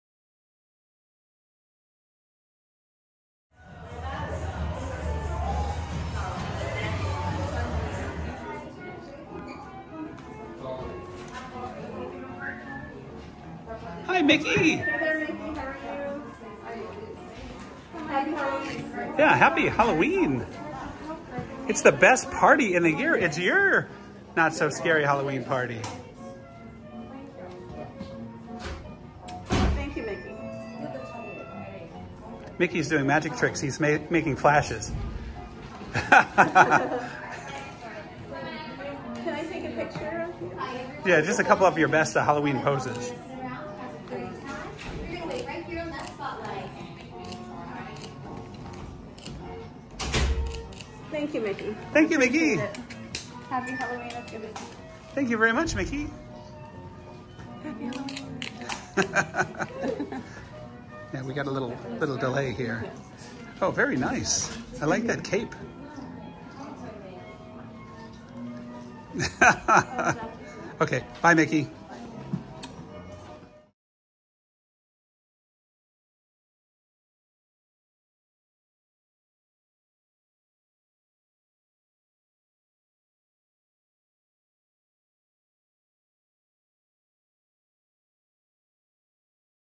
Mickey Mouse Meet and Greet at Mickey's Not-So-Scary Halloween Party 2023 - Town Square Theater
We met Mickey Mouse in his Halloween costume at Town Square Theater during Mickey's Not-So-Scary Halloween Party 2023!